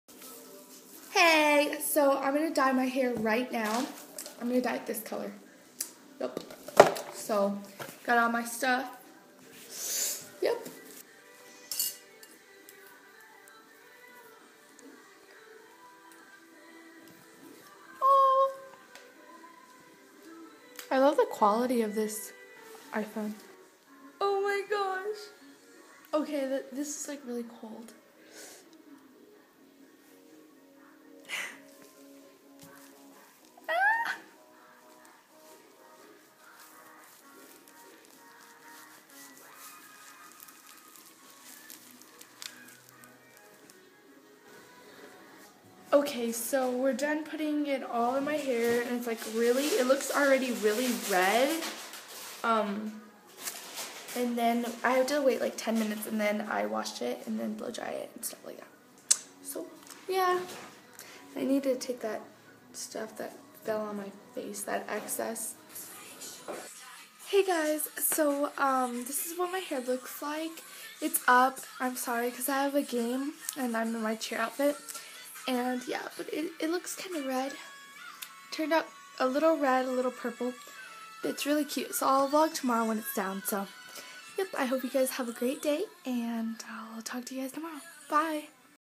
Day (8) dying my hair red/purple
Fun fact: this is the same color that Andrea Russet did when she made her, "dying my hair purple" 2 years ago.